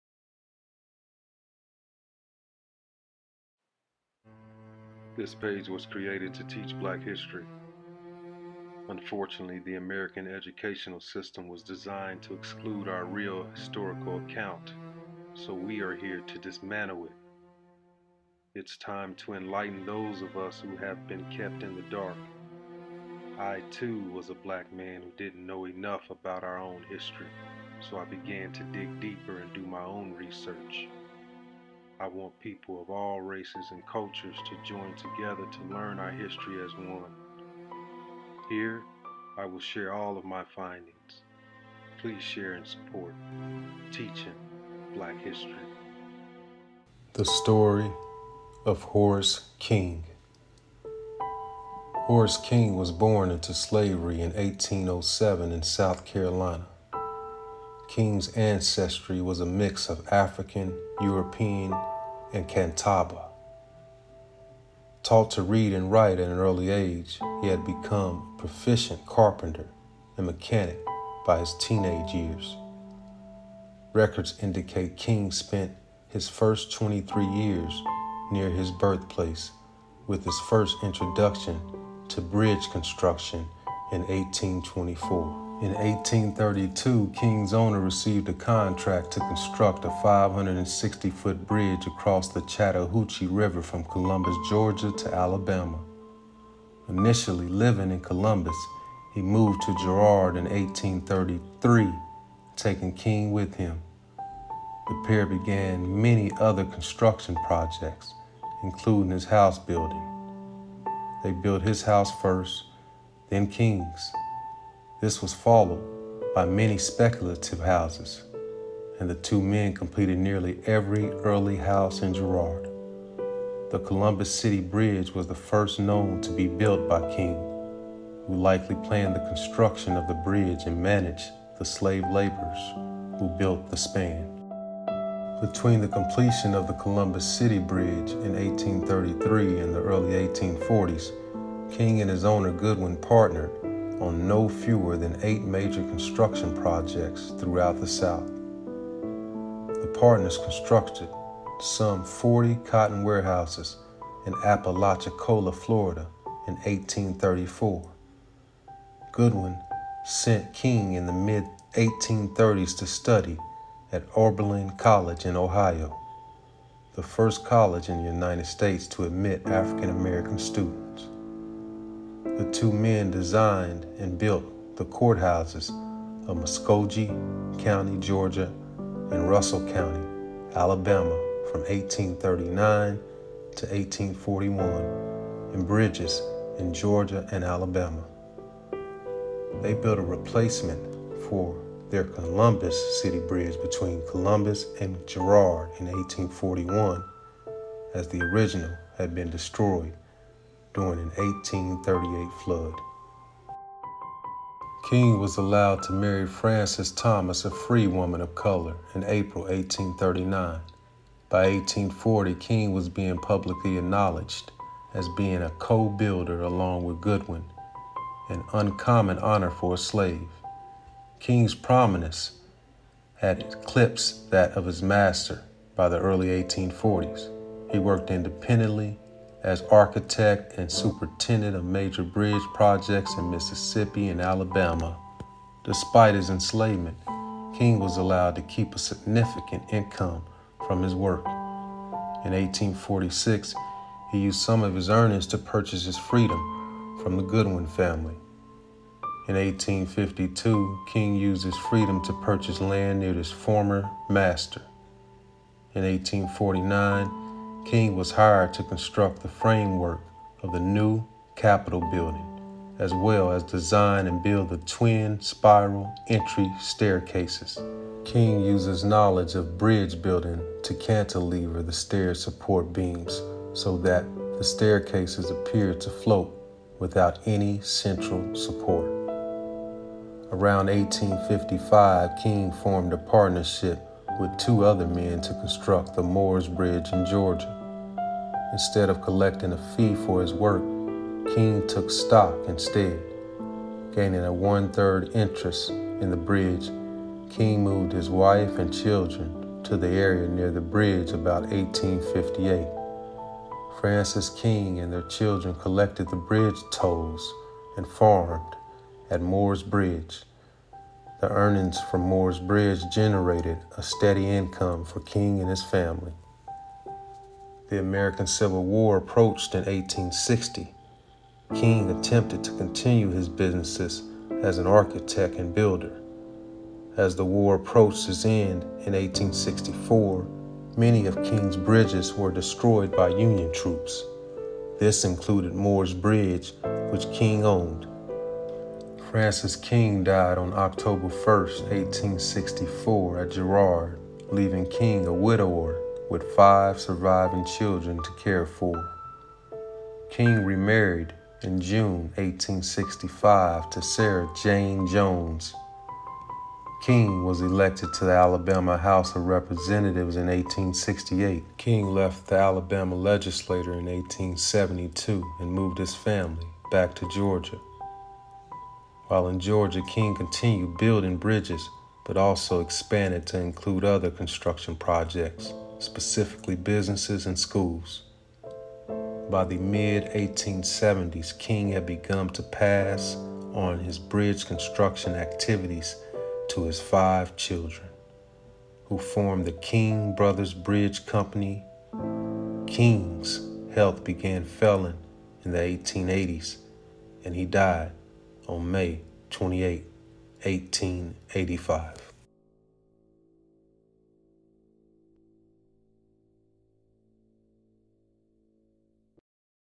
Horace The Bridge Builder King Story
- King and his master John Godwin worked as partners on major construction projects including bridges
- By 1860, he was one of the wealthiest free blacks in Alabama
- King travelled without any restrictions throughout the Deep South.
- First slave turned Architect
- He was a master engineer

Wikipedia contributors. (2022, March 12). Horace King (architect). In Wikipedia, The Free Encyclopedia.